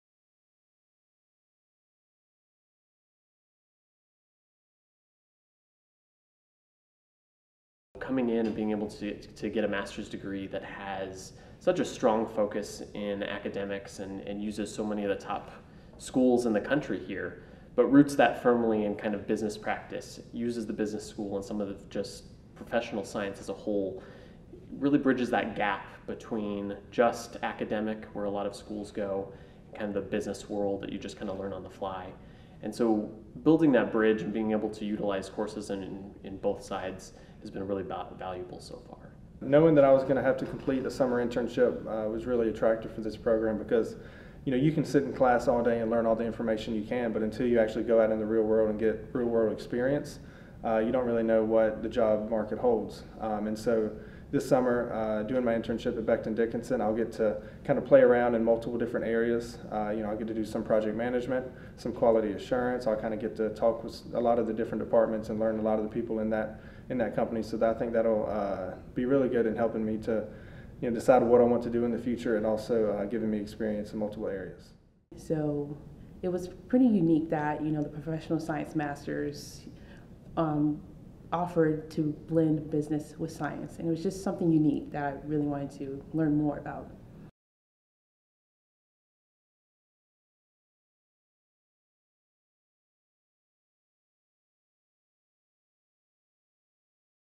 Why a Professional Science Masters?
Hear what current students find valuable about the PSM program.